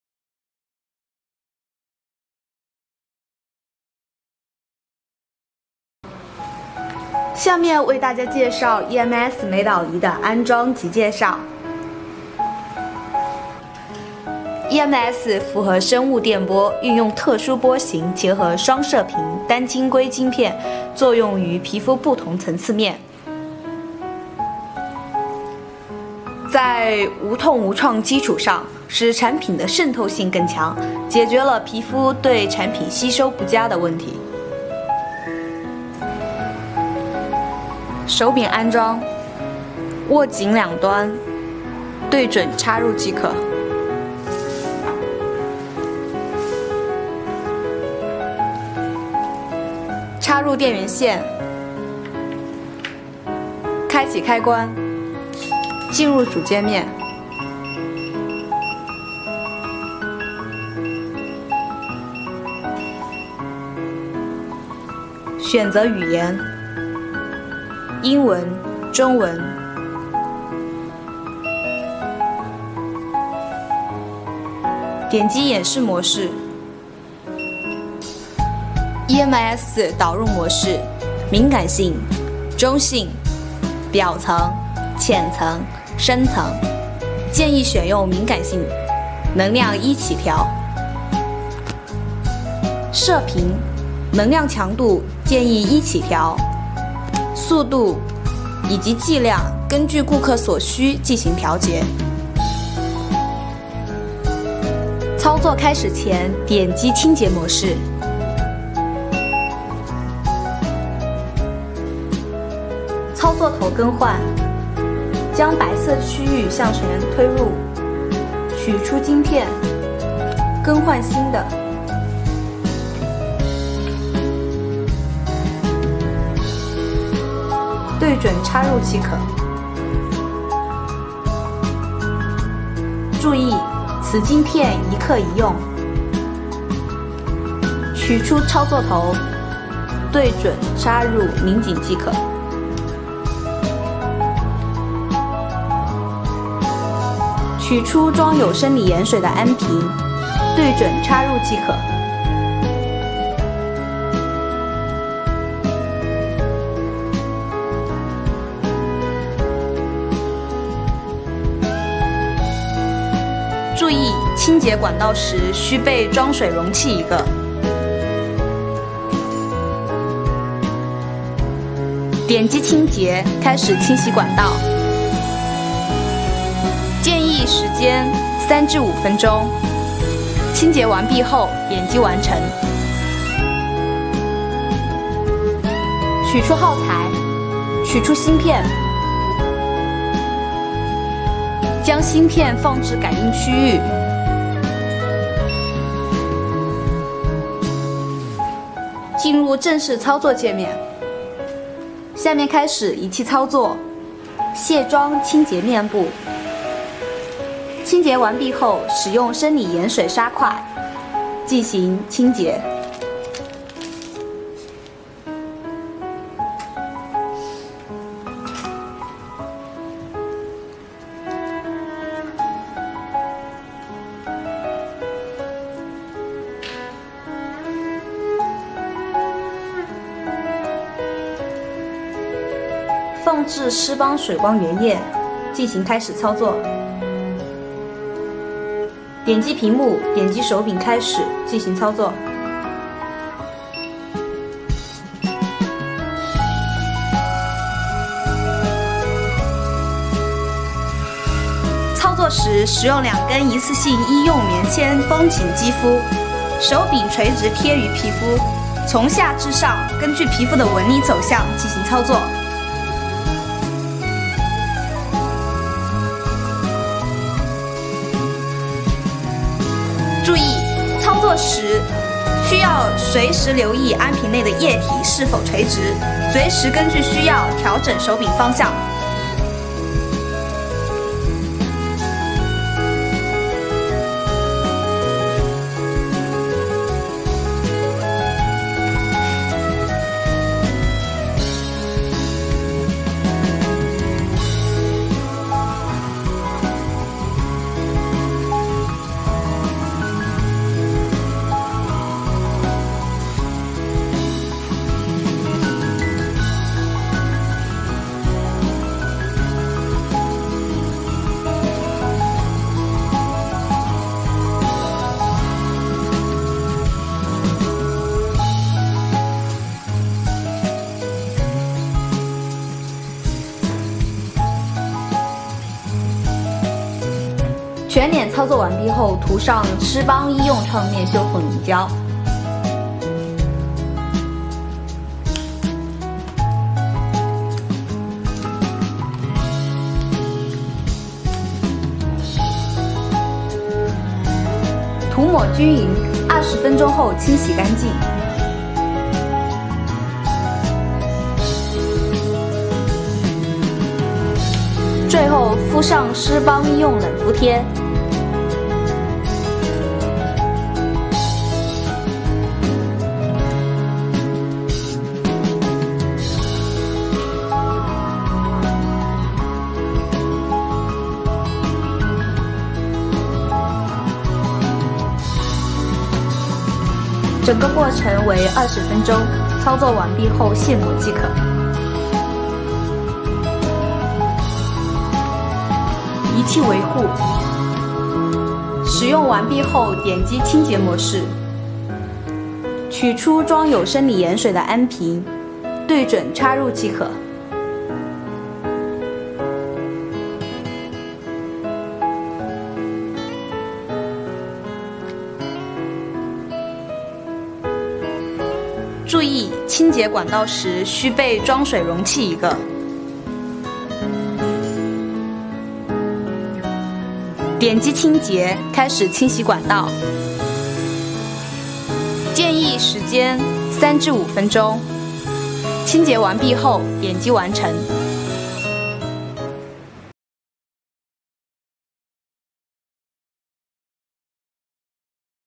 uangelcare NV-H9 EMS RF IC card control needle free mesotherapy machine
Nano injection mesotherapy gun adopts advanced processing technology to make an innovation on the injector. Different from mesotherapy guns that use needles, we use a microchip with multiple pins to deliver nutrition and essentials, which can be reached to subcutaneous layer under high pressures from the pistol.

Use the wispy needle of the micro crystal head to penetrate into the cuticle of skin, opening the pore of the epidermal layer of skin, injecting the active ingredient of nutrition. Therefore, it can repair aging skin and reborn collagen, remove wrinkles and spots, improve skin complexion and lift face.This Technology with RF radio frequency + EMS for skin lifting .